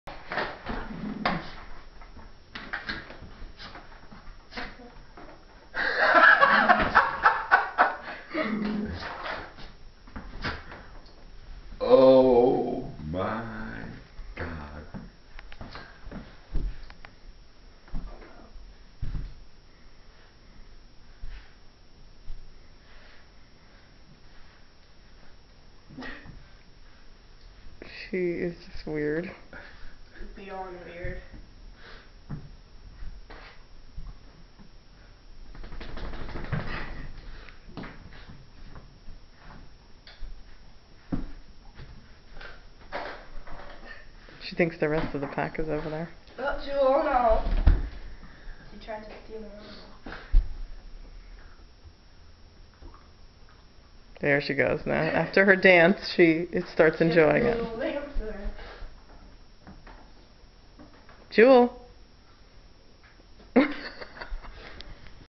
Doggie Christmas
My dog, Jewel's reaction to one of her Christmas presents!